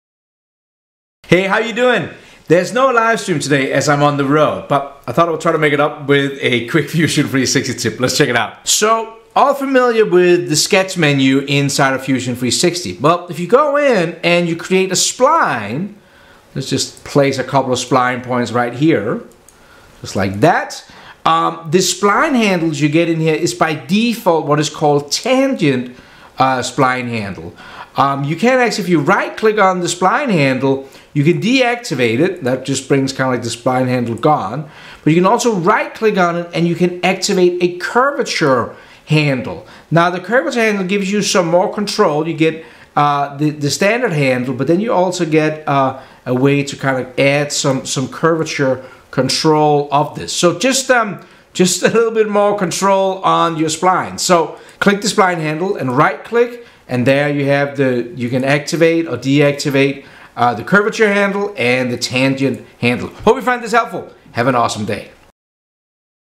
Fusion 360 Quick Tip — The Hidden Spline Function You did NOT Know!
No Livestream today, sorry, BUT I thought I could make it up with a quick Fusion tip. The Hidden Spline Function!!!